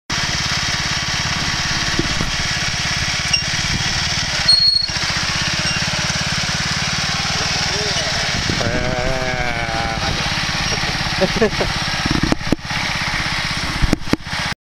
田植え機で隣の田んぼに乗り入れ!! 軽くスリリング(笑)
田植え機で、隣の田んぼに乗り入れる時の動画です。
苗を後ろに積んでいるので、前の部分が上がってしまうので、人が乗って重り代わりになります。今回の部分は大して高いところではなかったので、そんなでもないんですが、ちょっと高いところに上るときは、前に大人が二人乗ってもひっくり返りそうになるぐらいスリリングです。
実際に前の部分に乗ってみたんですが、手を放すと完全に落ちるし、ドキドキです(/・ω・)/